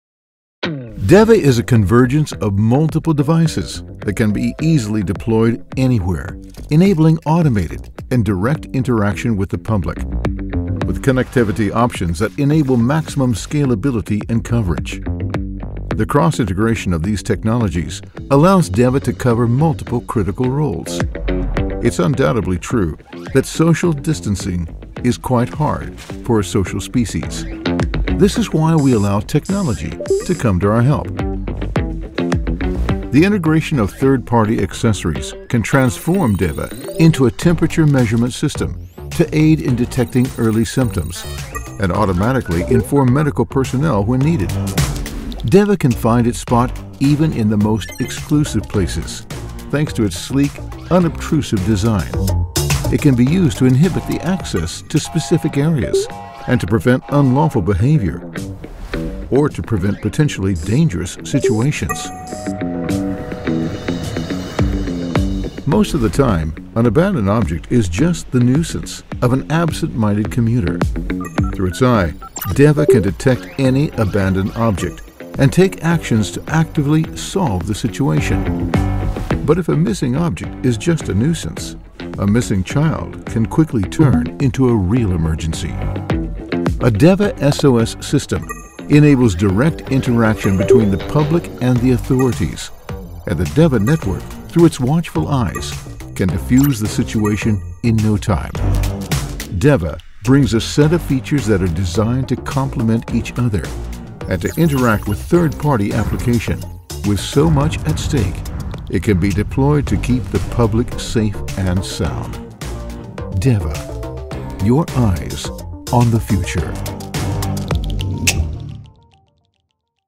Deva - COVID-19 Security Support
As an advanced multimedia device, Deva is a convergence of multiple devices that can be easily deployed anywhere, enabling automated and direct interaction with the public. Its camera, high powered speaker, and multiple sensors are designed to complement each other and to interact with third-party applications to offer several services spanning from safety, to information and entertainment.